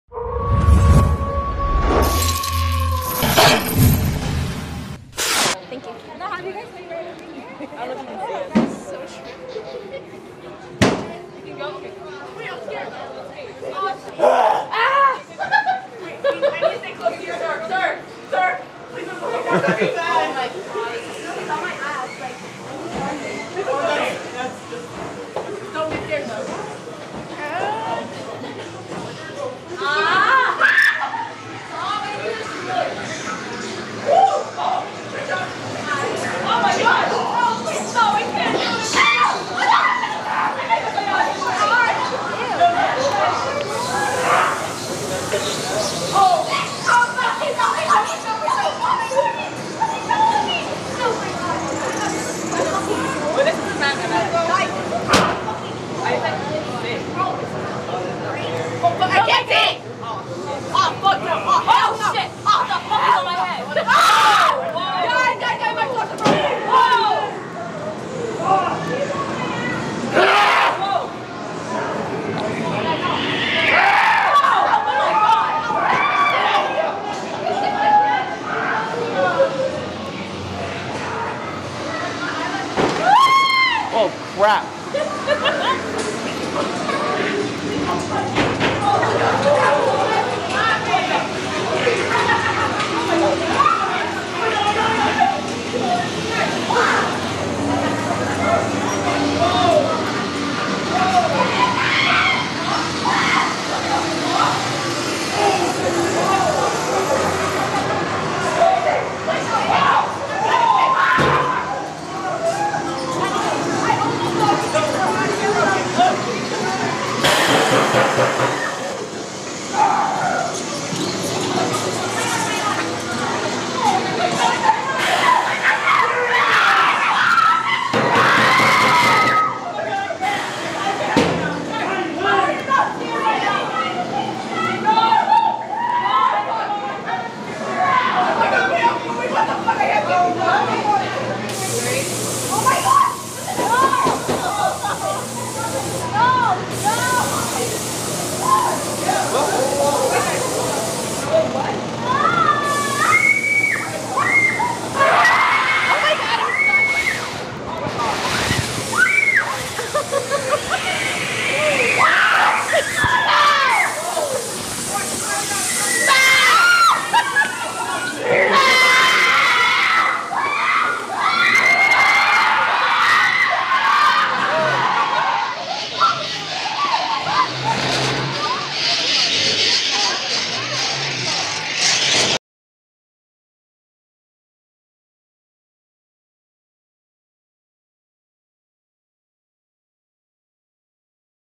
Sewer of Souls | Mic'd UP Walkthrough | Six Flags Magic Mountain | Fright Fest 2022
Mic'd UP Walkthrough with Rob and Friends! The Finale for the Six Flags Fright Fest Mic'd UP Walkthroughs is none of than Sewer of Souls!
(Description and Links for things)